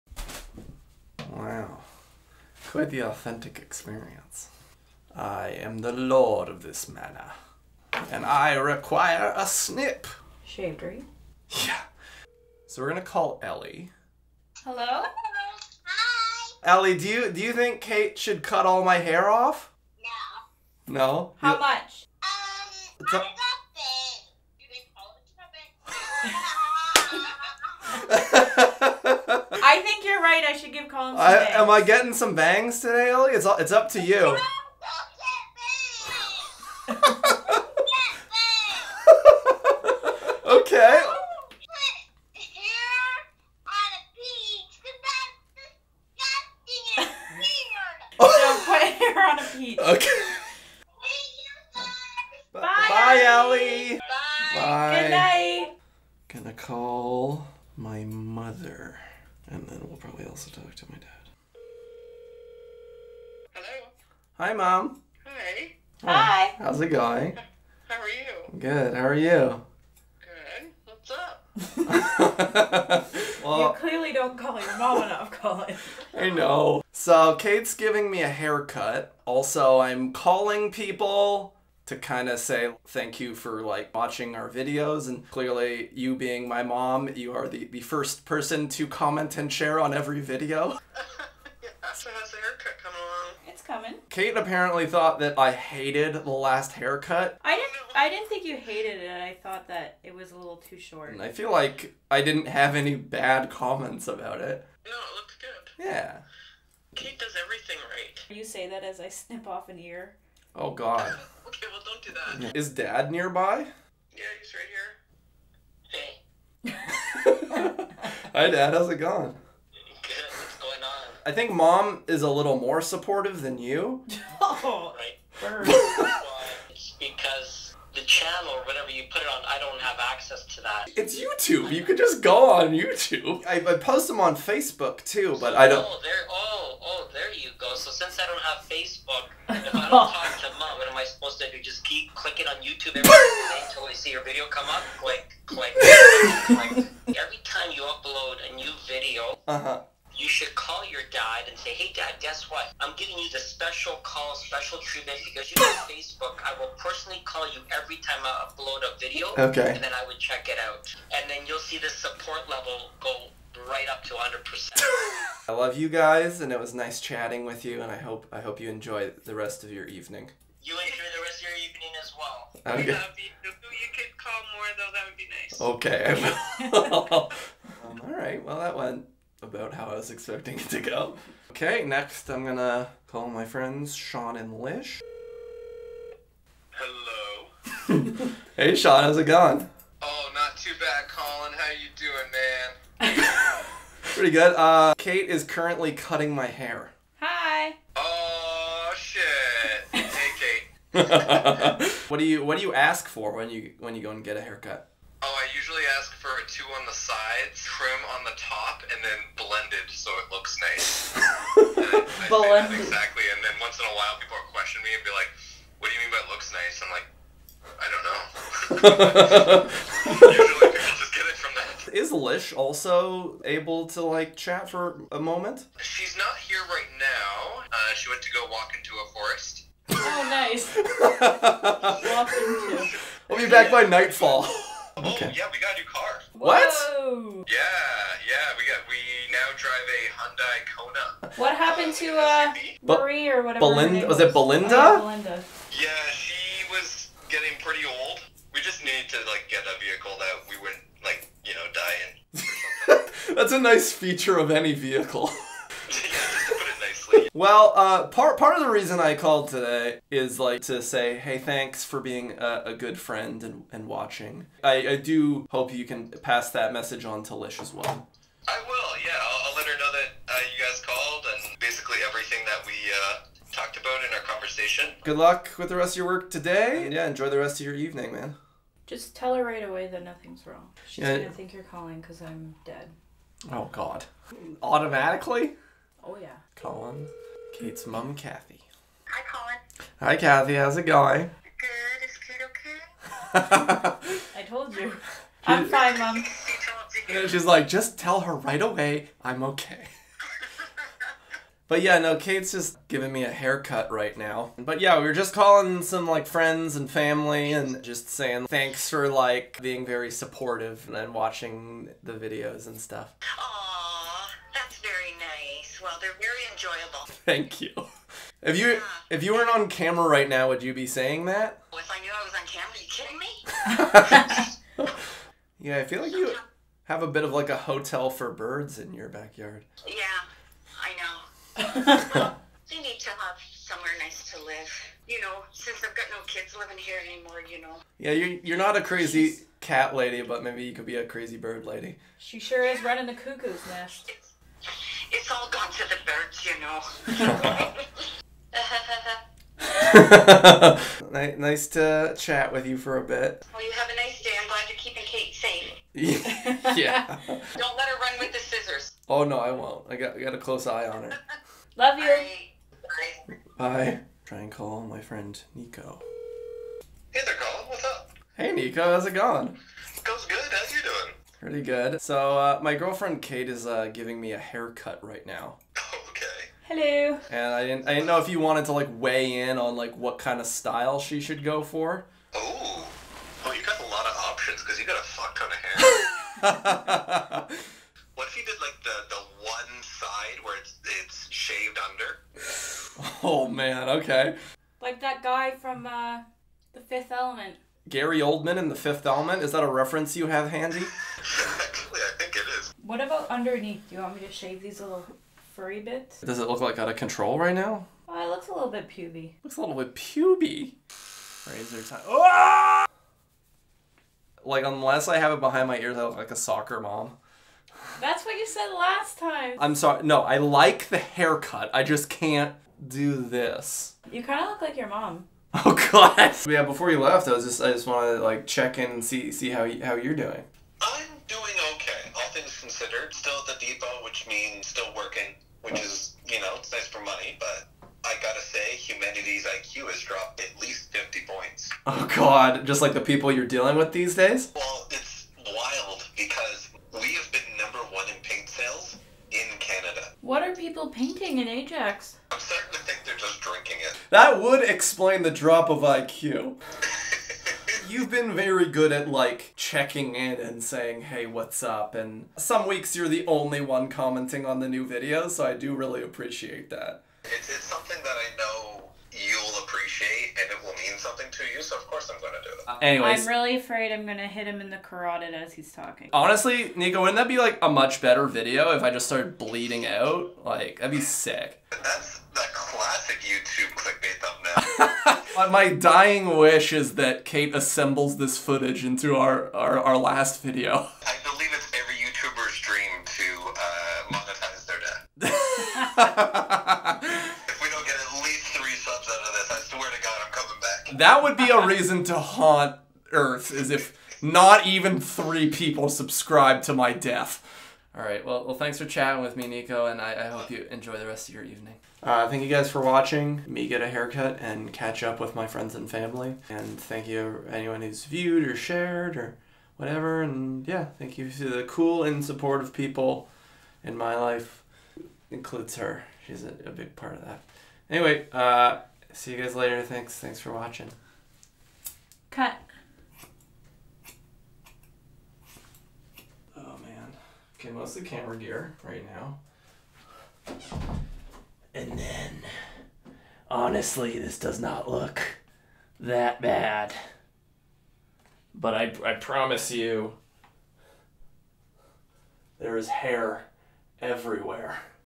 Getting a Haircut and Calling Friends and Family
So my hair was getting a tad bit mop like so I enlisted in the skills of master haircutter Kate. I also call my friends and family who have been nice enough to watch these videos and go "hey that was pretty good."